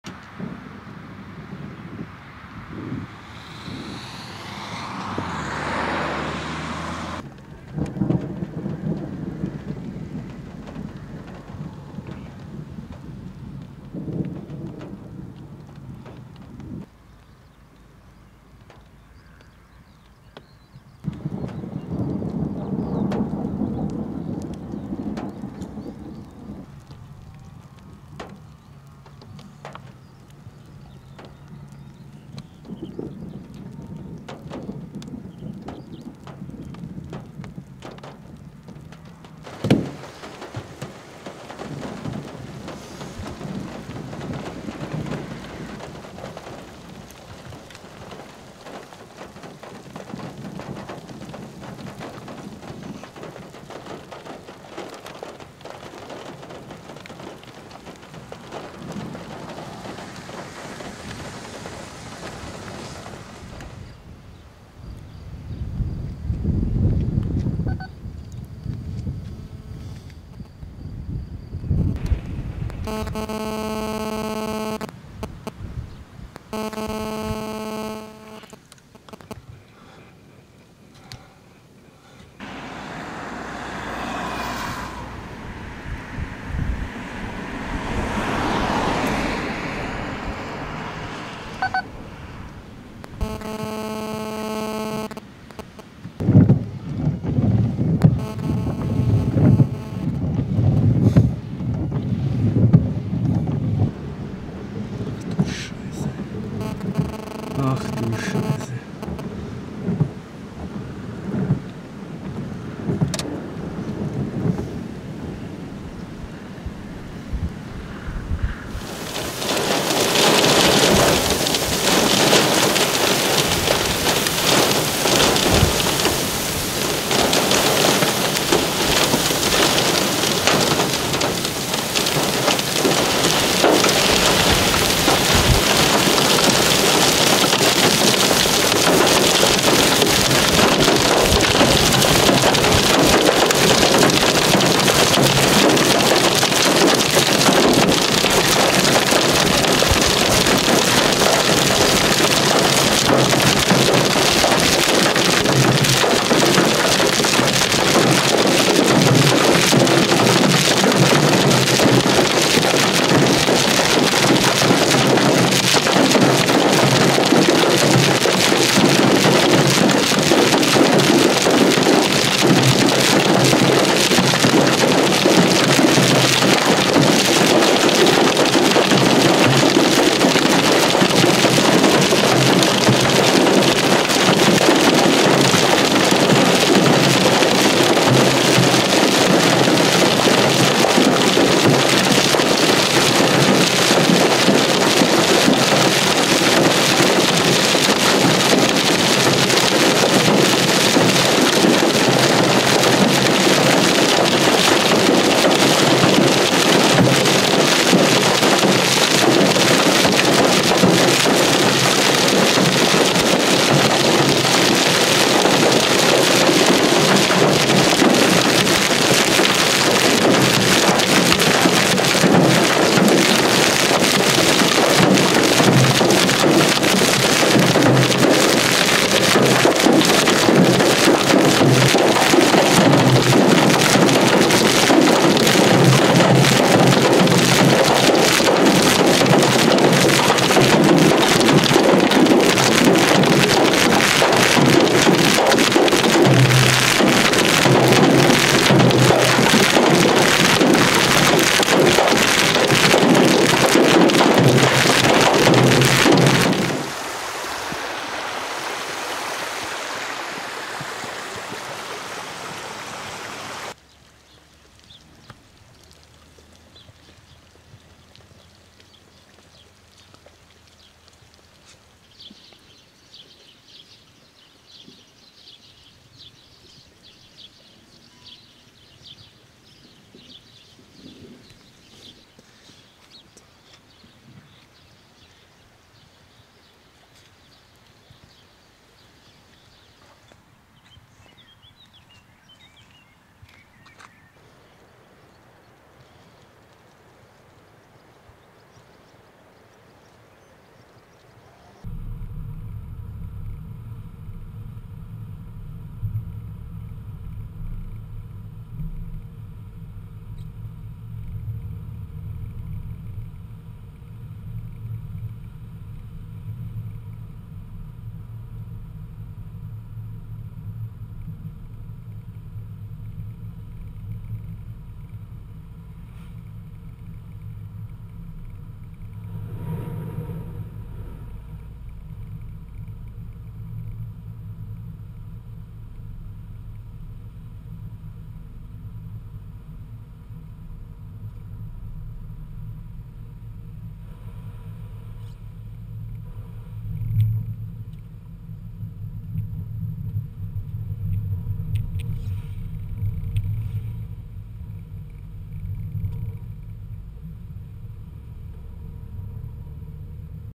Heftiger Hagelsturm, Wessobrunn in Oberbayern, 28.05.2016
Einer mit der heftigsten Hagelunwetter die ich je erlebt habe. Anfänglich viel recht dichter, 5 cm großer Hagel mit schweren Sturmböen. Ich dachte, gleich schlägt es mir ne Scheibe im Wagen ein. Aber zum Glück alles heil geblieben.